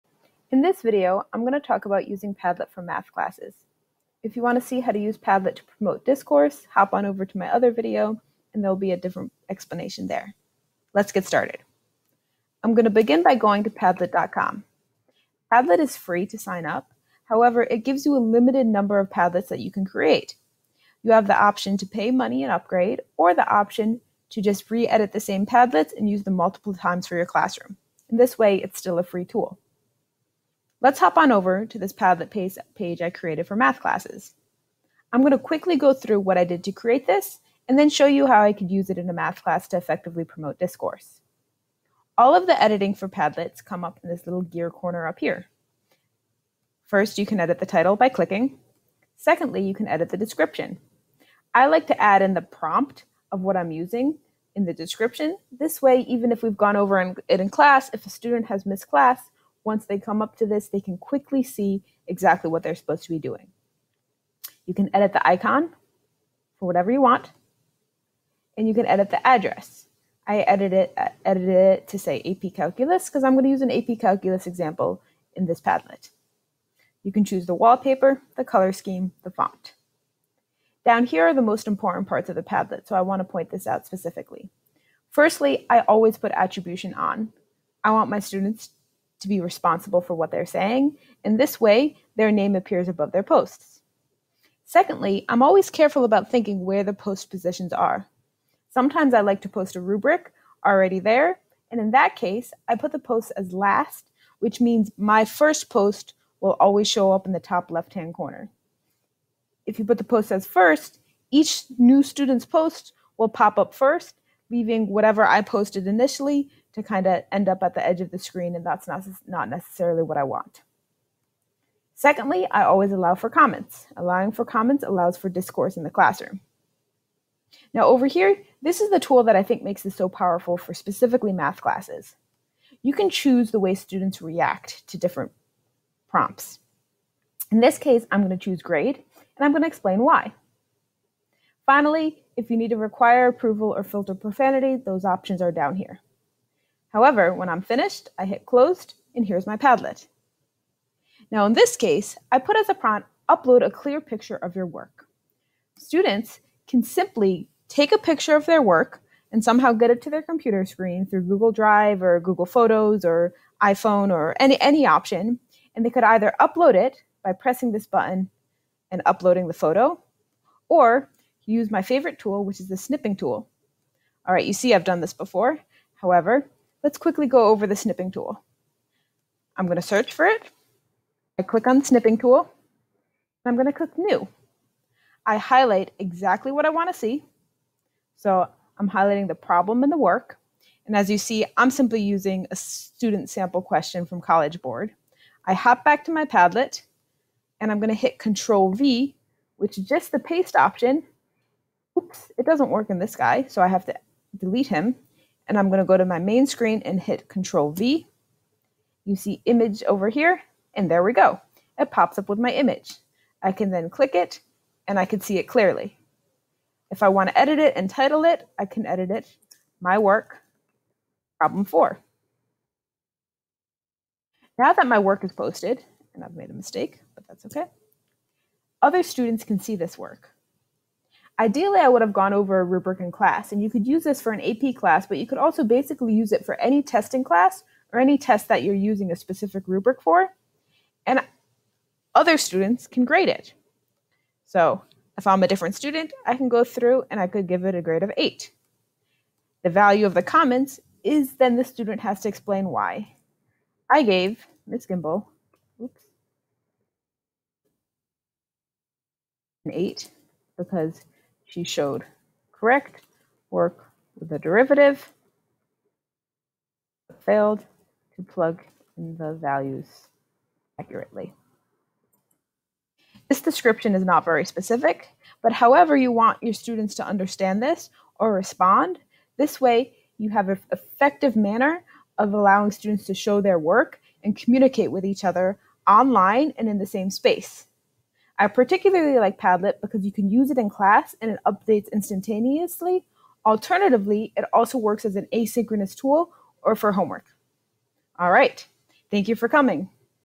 Using Padlet for Discourse in Math Classes
This is a video which explains how to use Padlet to allow students to post their work and respond to others. This is particularly powerful for teaching students how to carefully use rubrics, identify errors in other's work, and clearly explain their ideas. It is effective for use in class and as homework or during an asynchronous lesson.